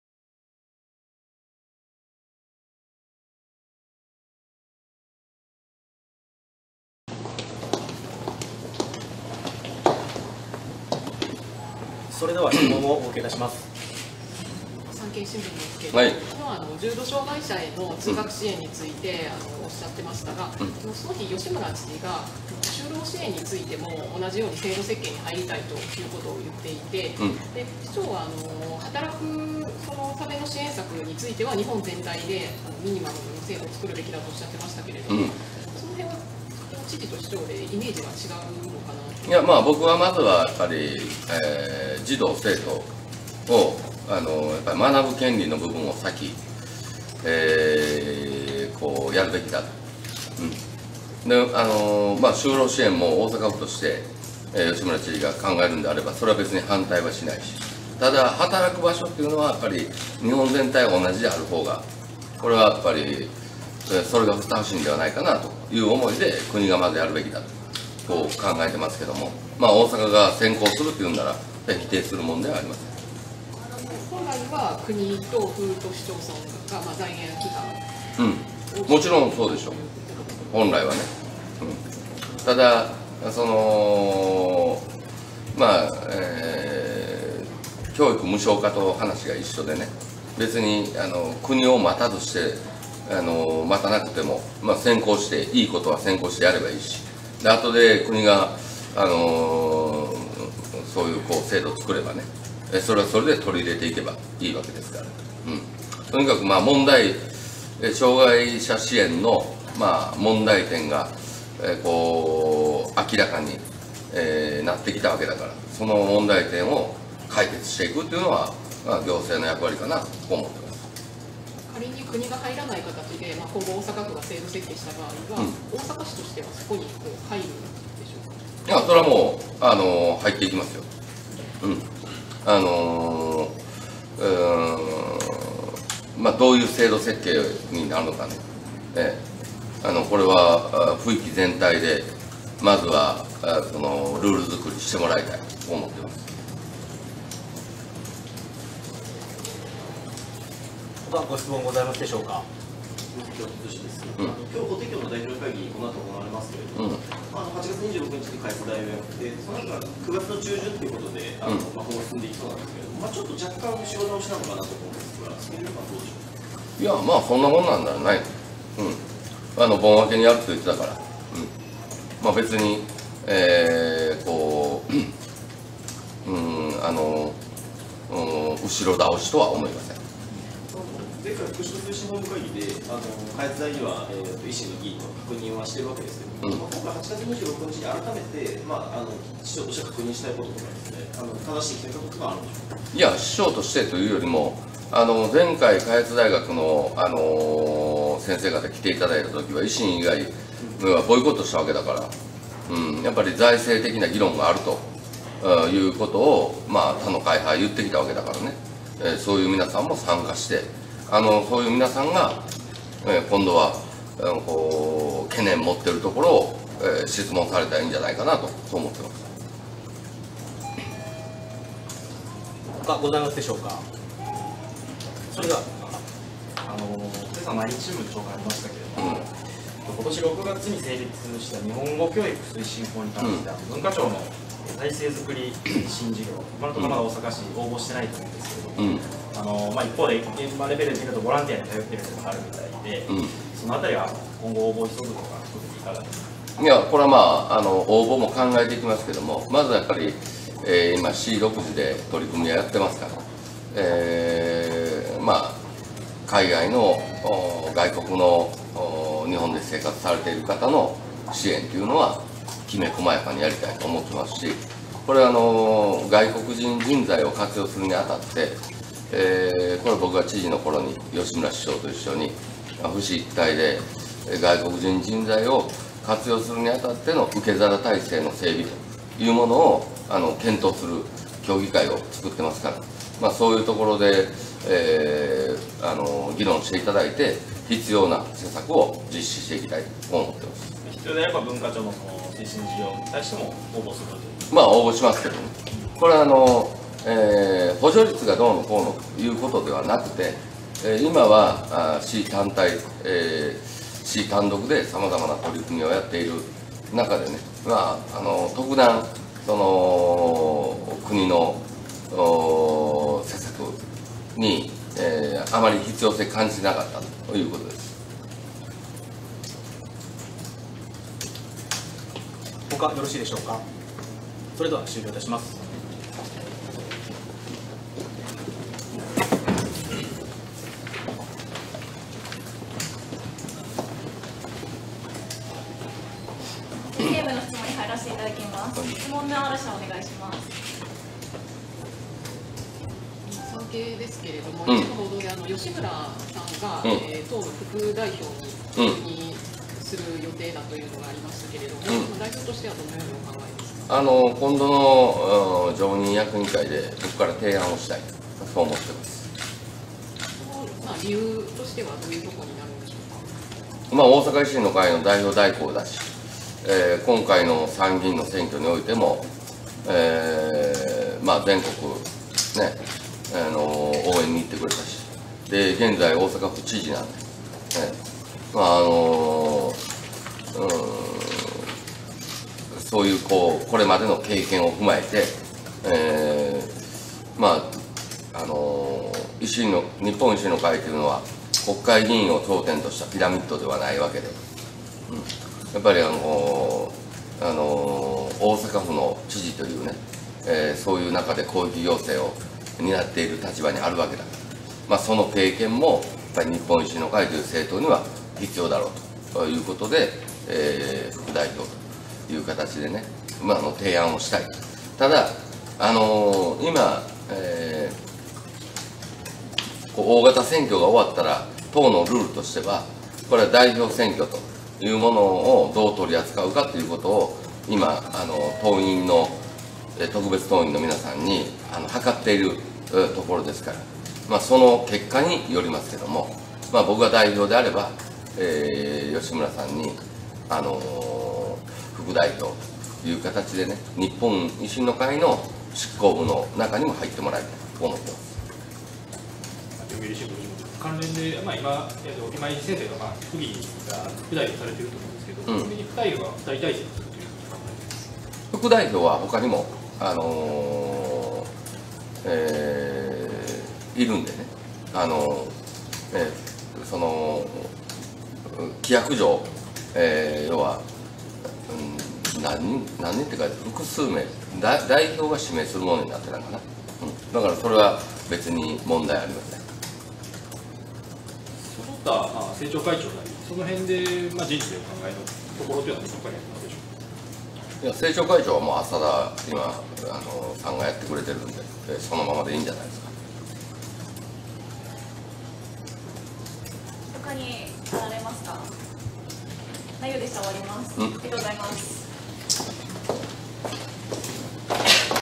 松井市長囲み会見 (2019.08.08) ■重度障害 就労支援について ■法定協議会開催について ■日本語教育支援事業について ■吉村知事の党副代表提案について
令和元年8月8日 松井市長 囲み会見
・重度障害 就労支援について
・法定協議会開催について
・日本語教育支援事業について
・吉村知事の党副代表提案について